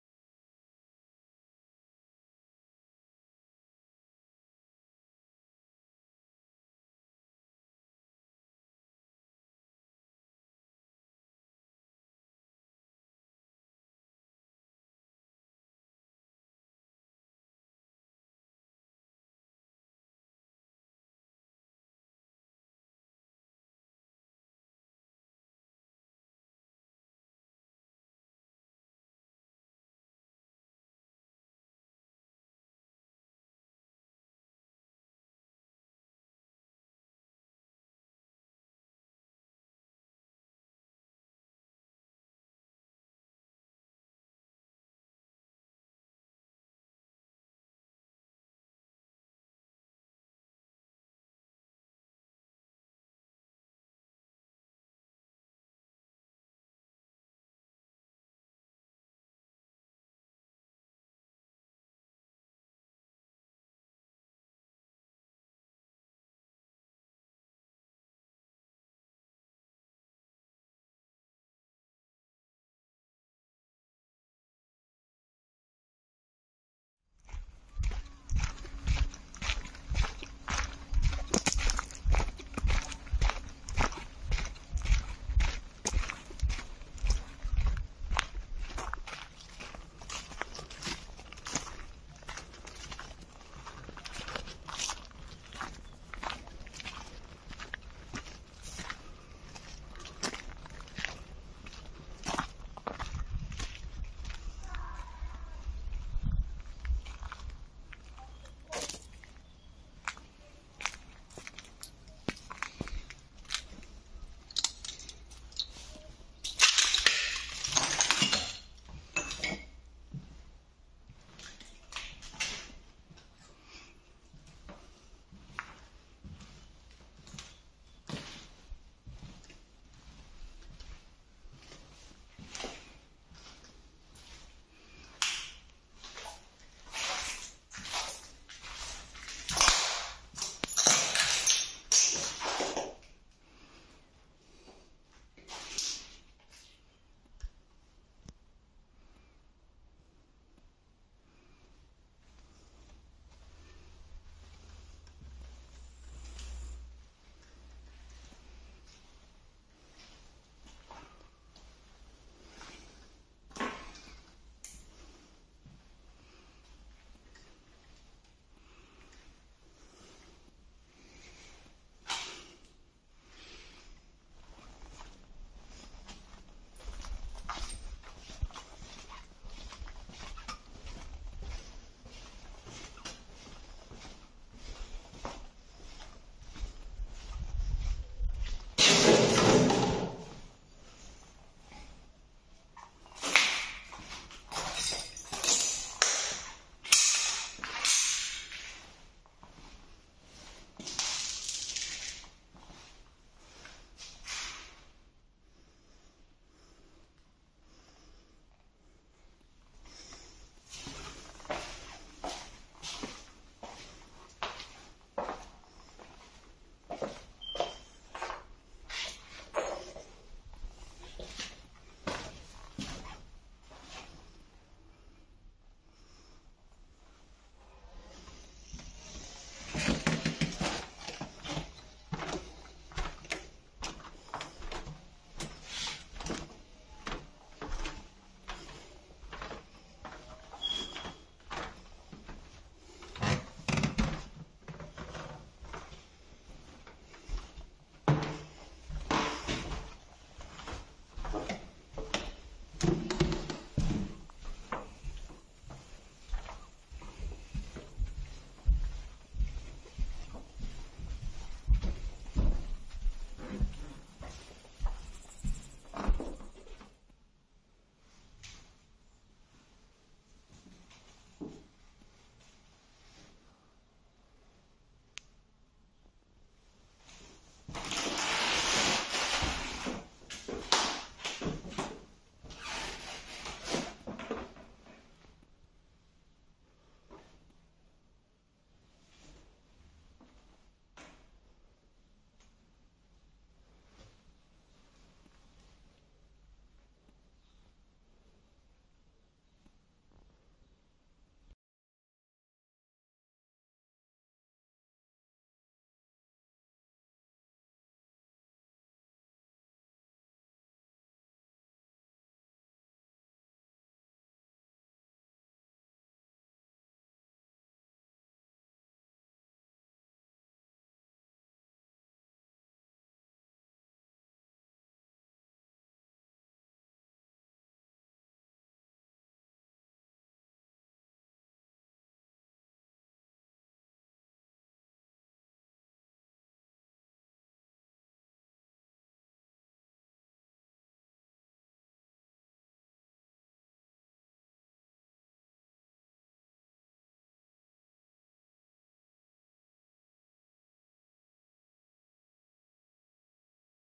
Das verlassene Hotel am See
Hier buchen schon lange keine Gäste mehr!
1980 im Schlaf versunken und verlassen.
Das alte Hotel am Badesee - weitgehend frei von Vandalismus und mitten im Nirgendwo.